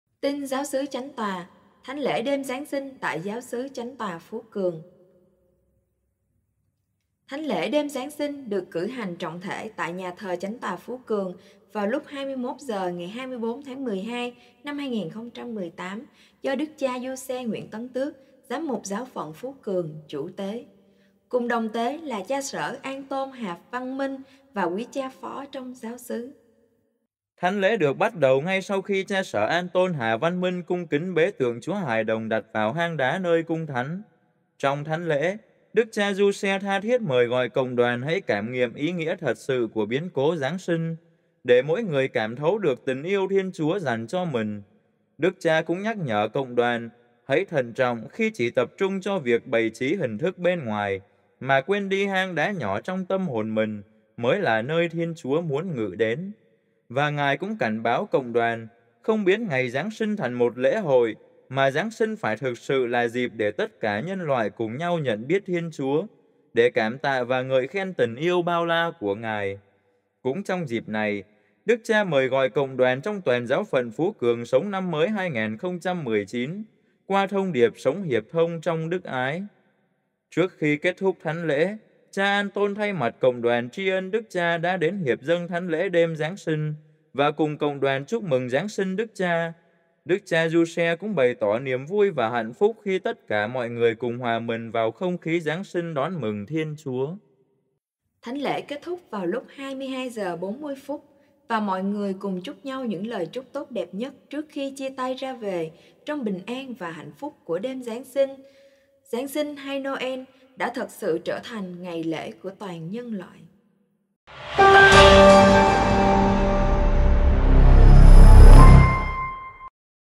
TIN GIÁO XỨ CHÁNH TÒA: Thánh Lễ Đêm Giáng Sinh Tại Giáo Xứ Chánh Tòa Phú Cường
Trích: ĐIỂM TIN TRONG TUẦN GIÁO PHẬN PHÚ CƯỜNG 24.12 ĐẾN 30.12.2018